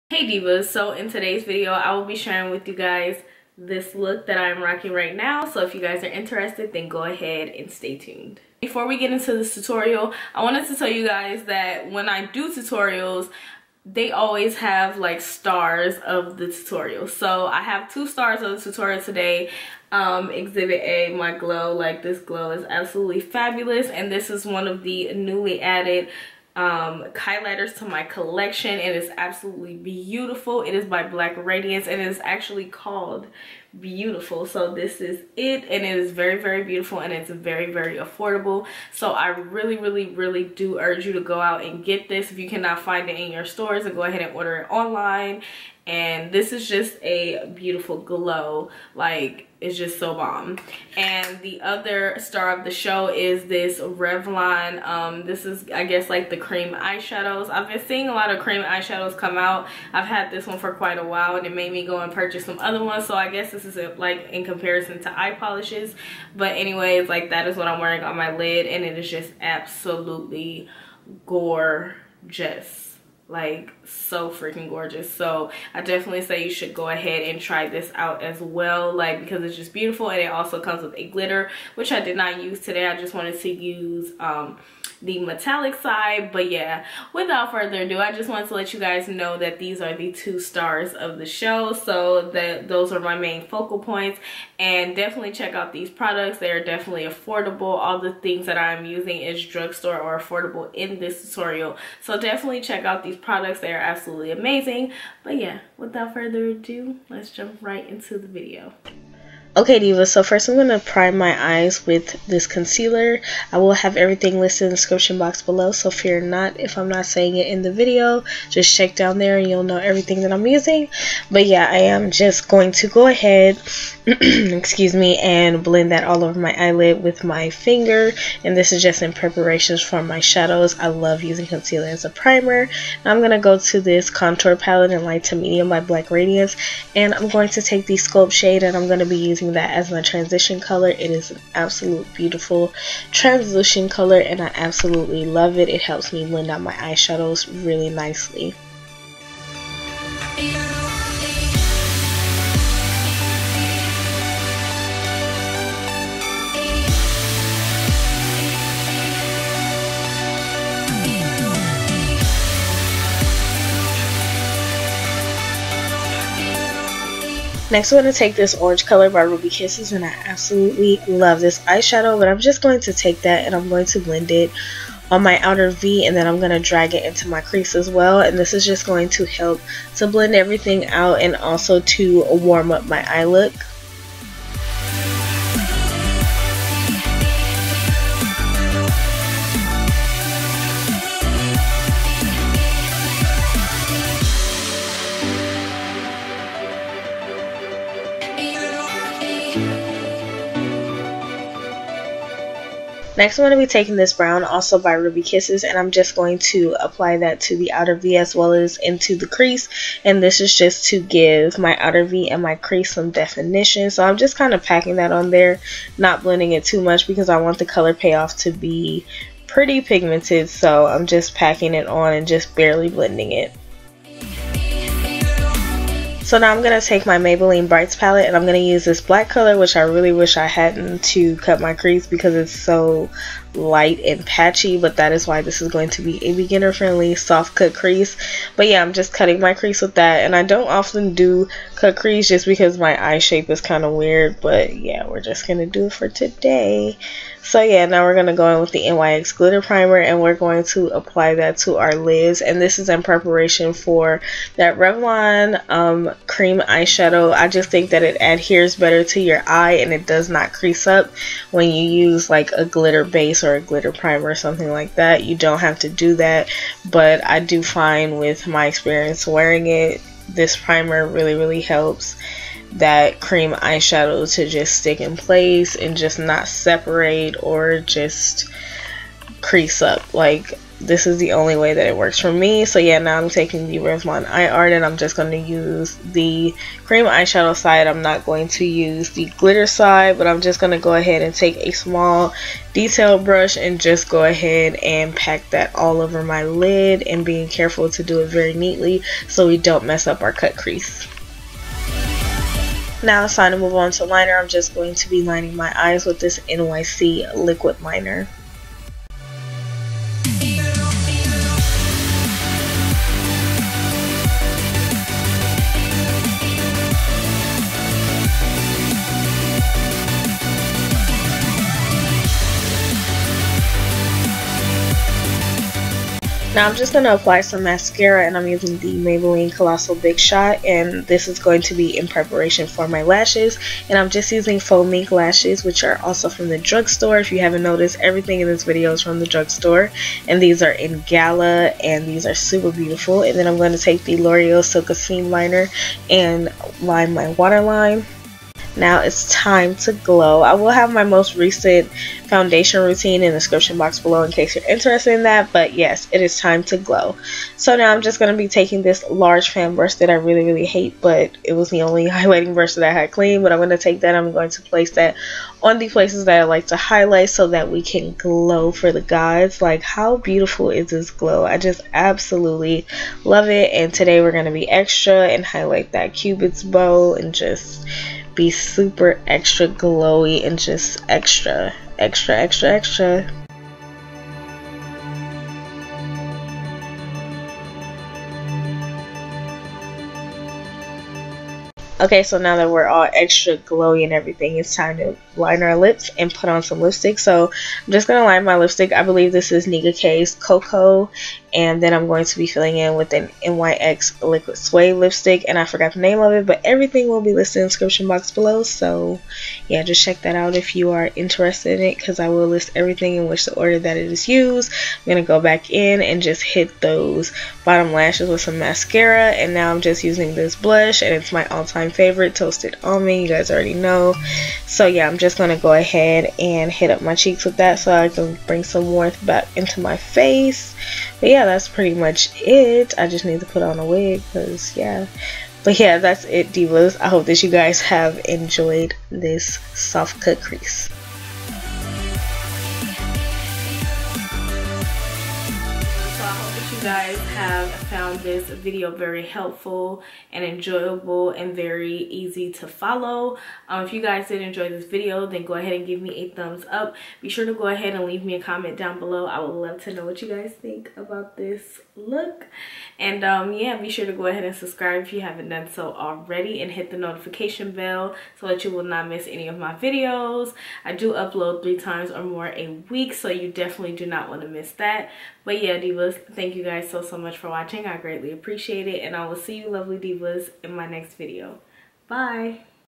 EASY CUT CREASE EYESHADOW TUTORIAL 2017
Maybelline better skin concealer medium deep
black radiance light to medium sculpt shade
 single eyeshadow  geranium
 single eyeshadow dark brown
Maybelline the brights palette black eyeshadow
nyx glitter primer
Revlon cream eyeshadow 100 topaz twinkle
nyc liquid liner in black
Maybelline colossal big shot mascara
kiss lashes in gala
loreal eyeliner black
black radiance baked bronzer in beautiful
nicka k lip liner in cocoa
nyx liquid lipstick sand
black radiance blush in toasted almond

I accept both compliments & criticism graciously knowing that it takes both sun & rain for a flower to grow.